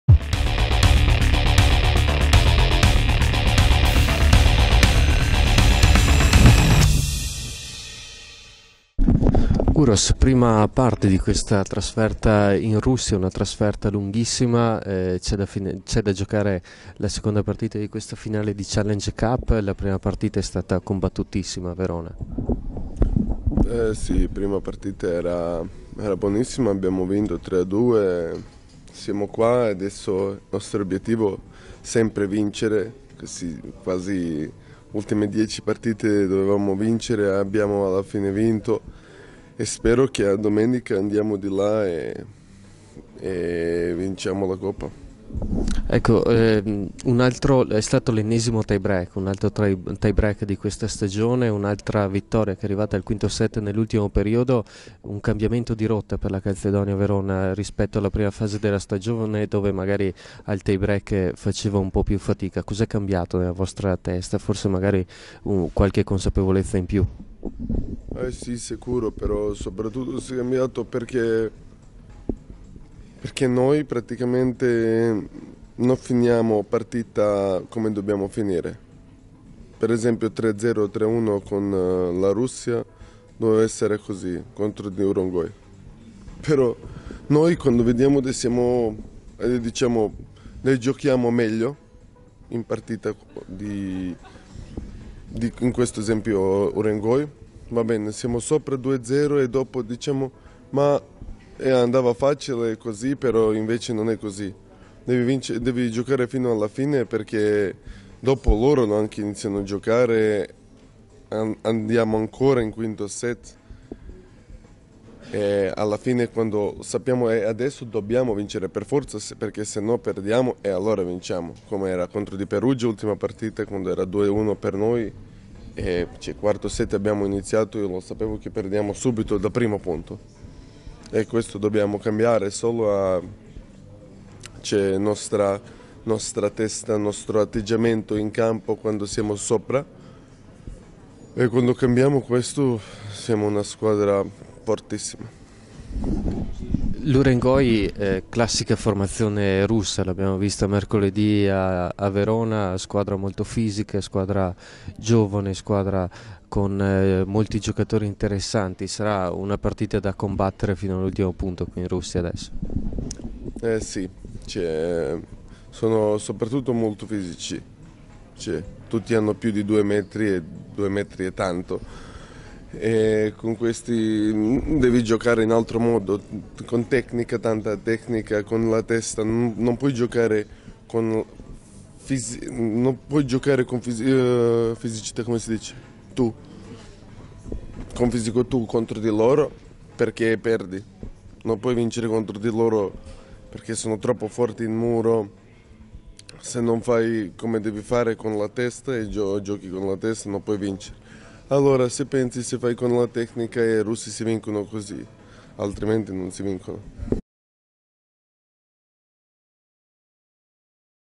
Kovacevic: "Prima partita combattuta, l'obiettivo è sempre quello: vincere"
Uros Kovacevic,. schiacciature della Calzedonia Verona, ha commentato la vigilia del match contro i russi del Fakel Novy Urengoy.